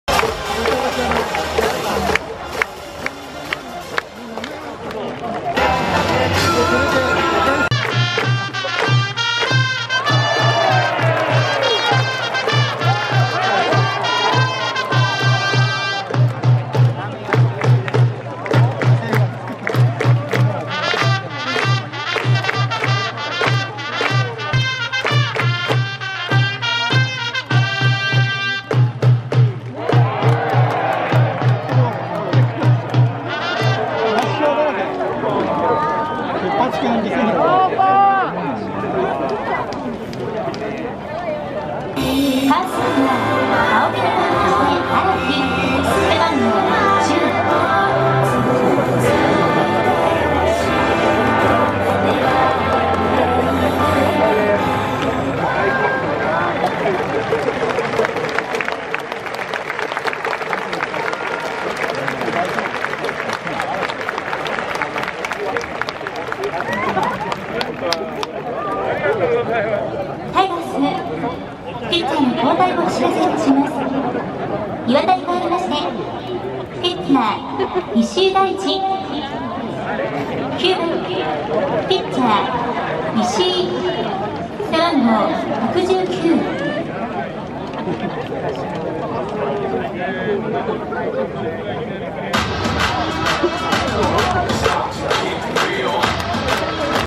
岩田一軍復帰登板｡青木と対戦｡
岩田が復帰登板。青木と対戦しましたが、デッドボールを与え、降板。残念でした。
2021-7-7 ヤクルト対阪神タイガース 神宮球場

マルテがおちゃめ！塩見が出塁した時､ボールボーイの頭をポンするw

阪神藤浪がヤクルト宮本にデッドボール｡宮本が落ち着いてから､宮本を見てちゃんと謝る藤浪｡